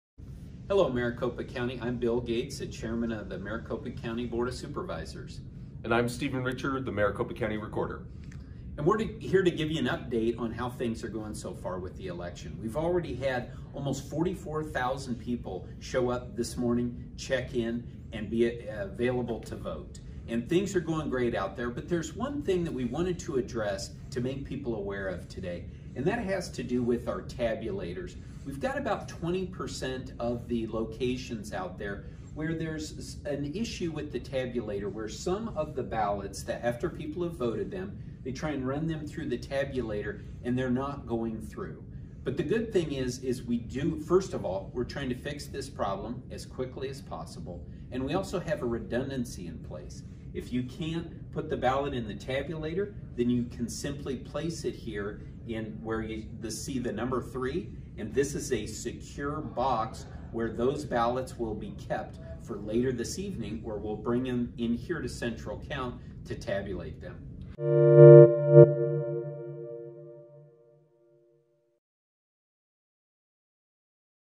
Election Day: Arizona's Maricopa County Sees Vote Tabulator Glitches
In Arizona’s Maricopa County, elections officials have flagged issues with tabulators in about 20% of polling locations. If the tabulator doesn’t read a ballot, voters can “insert their ballot in the secure slot on the ballot box where it will be counted at the Tabulation and Election Center,” the county elections office said in a statement.

Those ballots will be centrally tabulated later this evening, officials said, similar to how the country handles early voting -- but without the need to further confirm identification, because ballots dropped at polling places today are already effectively signature-verified, officials said.

“Technical staff are working to resolve an issue with tabulators and investigating the cause,” the statement said.